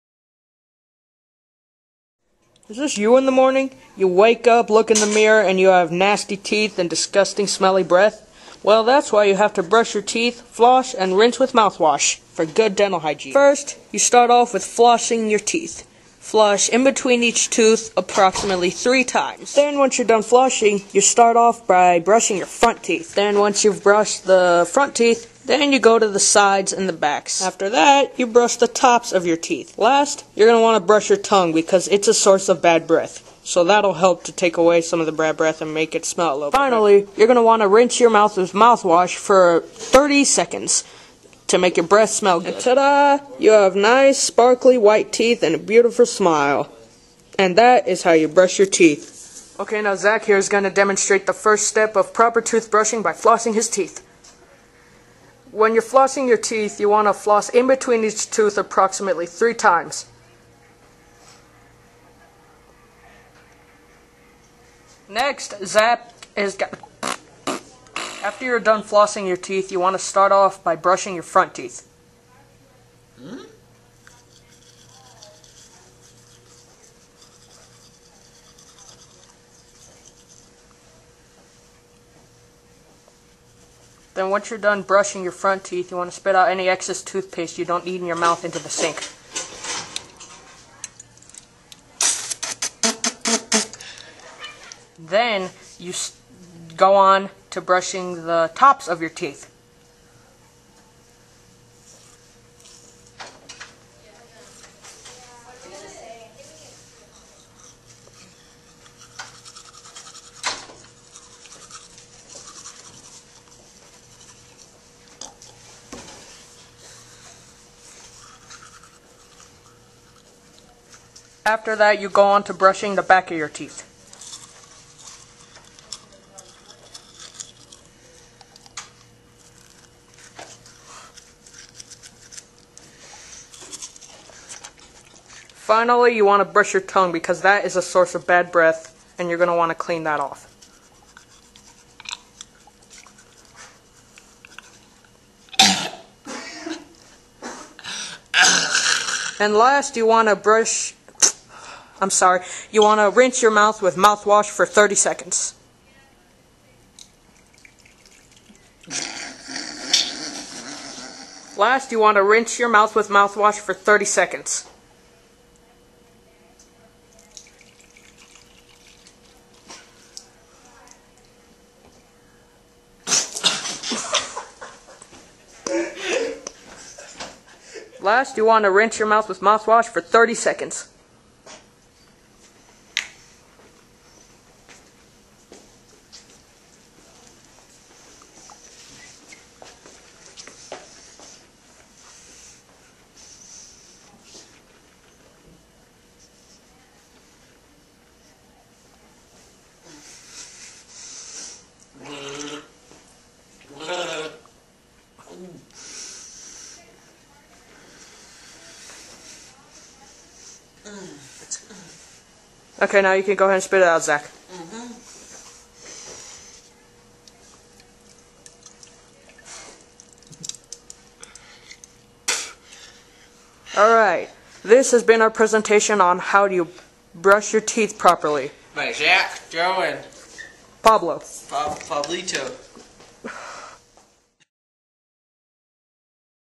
How to brush your teeth properly : School Project
This is a video project that me and a few other guys did at our school for our medical class explaining good dental hygiene. I suggest that you watch it, because a lot of you guys dont really know how to take good care of your mouth. No offense. I was the camera man for this project, and narration was by me.
Hope You guys enjoy it! bloopers includes.

Remember to rate and comment.